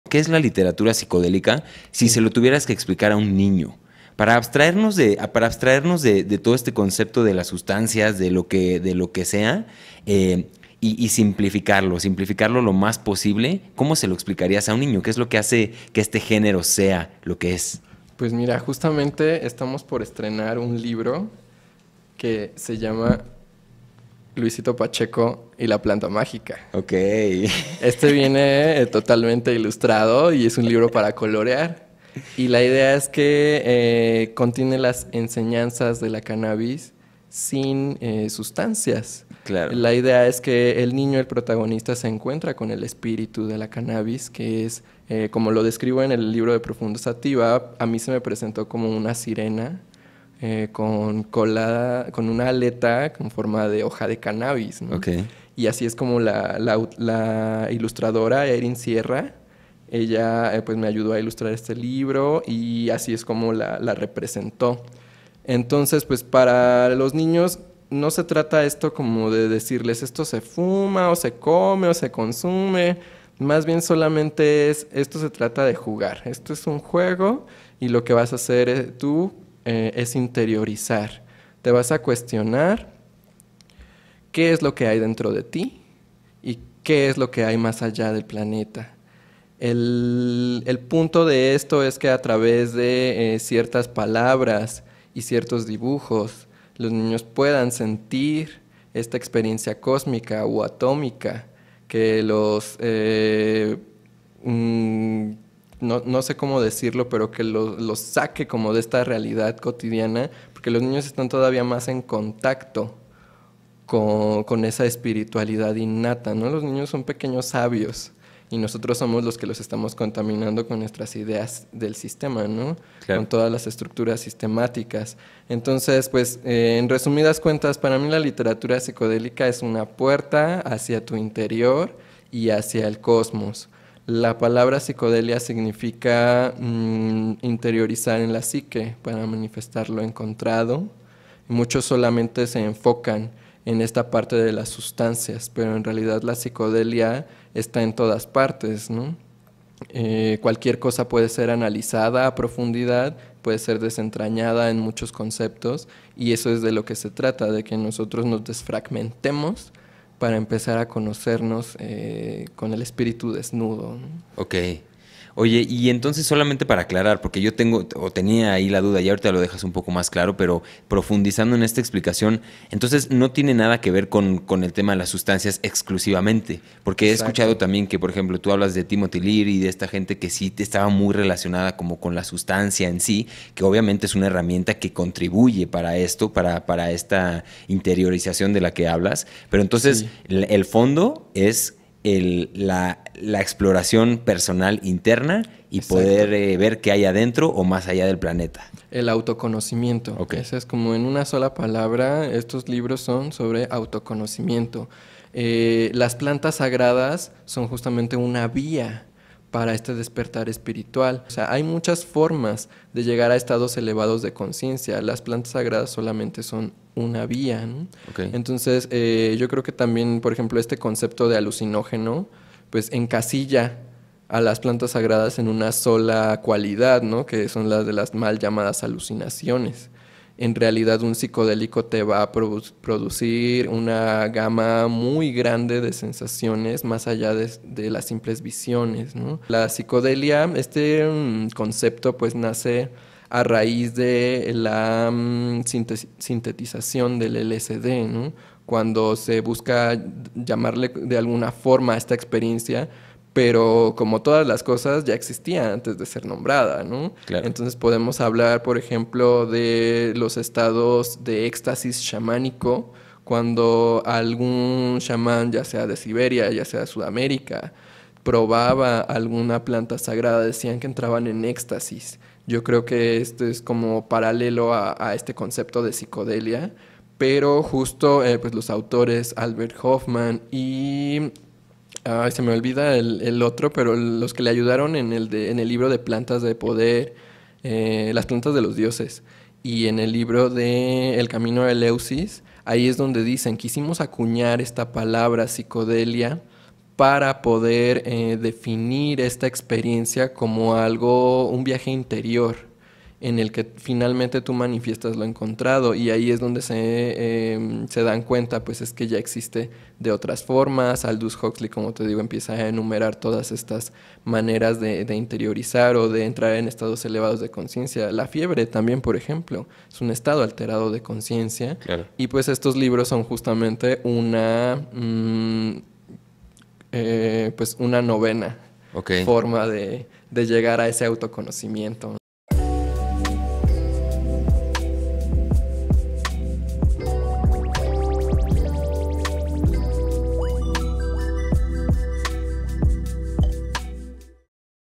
¿Qué es la literatura psicodélica? - Con los Pies en la Tierra
Zadkiel explica qué es la literatura psicodélica y cómo de relaciona con las diferentes vías del despertar de consciencia.